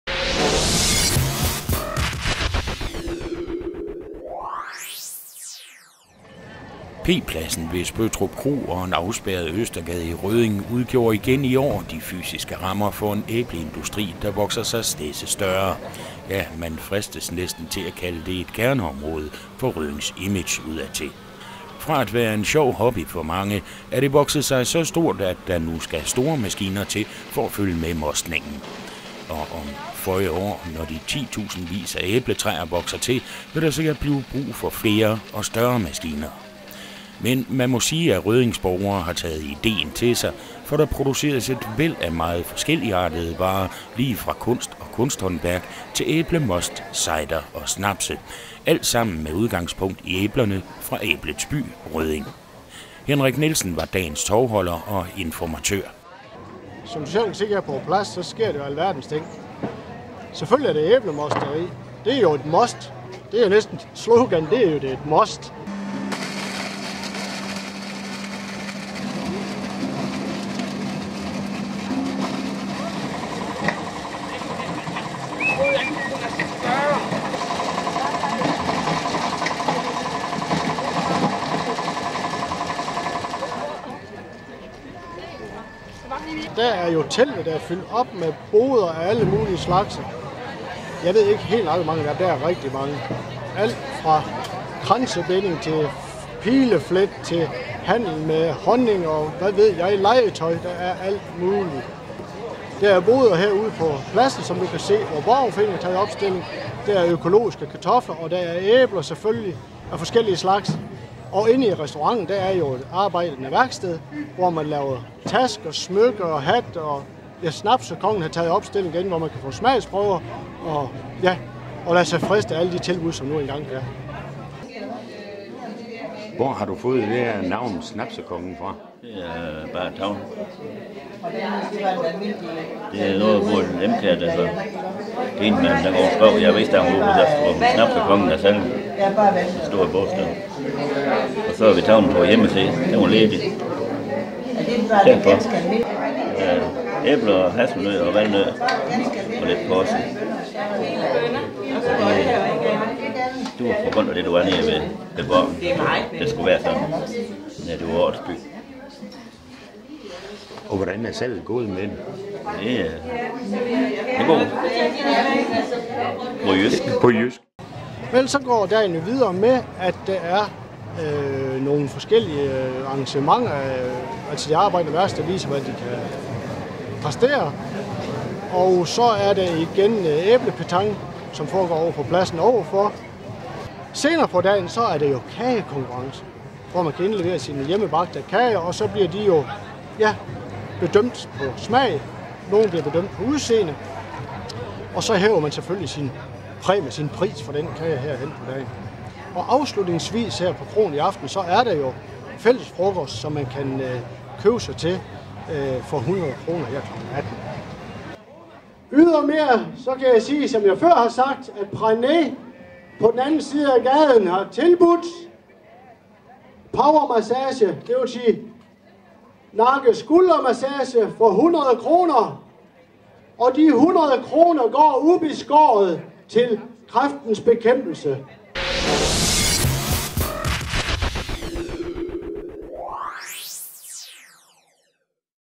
Æbledag i Rødding 2012
P-pladsen ved Spøttrup Kro og en afspærret Østergade i Rødding udgjorde igen i år de fysiske rammer for en æbleindustri der vokser sig stedse større. -- ja man fristes næsten til at kalde det er "kerne"-område for Røddings image udad til. Fra at være en sjov hobby for mange, er det vokset sig så stort at der nu skal de store maskiner til for at følge med mostningen. Og om føje år når de titusindvis af æbletræer vokser til, vil der sikkert blive brug for flere og større maskiner.
Men man må sige at Røddings borgere har taget idéen til sig, for der produceres et væld af meget forskelligartede varer, lige fra kunst og kunsthåndværk til æblemost, cider og snapse -- alt sammen med udgangspunkt i æblerne fra Æblets by -- Rødding.